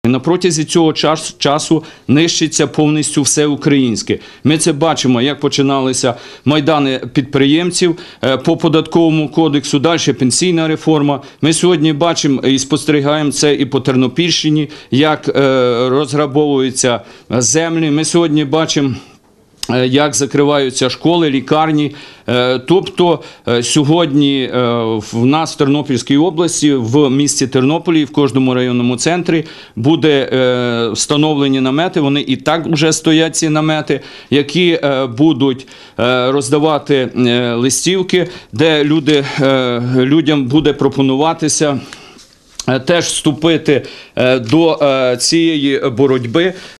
Деревляний заявив про акцію протесту
Про безстрокову акцію протесту сьогодні на прес-конференції заявив народний депутат України, голова Тернопільської обласної організації всеукраїнського об'єднання "Батьківщина" Василь Деревляний. Акція, за словами нардепа, паралельно розпочалась по всій Україні. У кожному районному та обласному центрі стоятимуть намети від об'єднаної опозиції, де людям роздаватимуть листівки.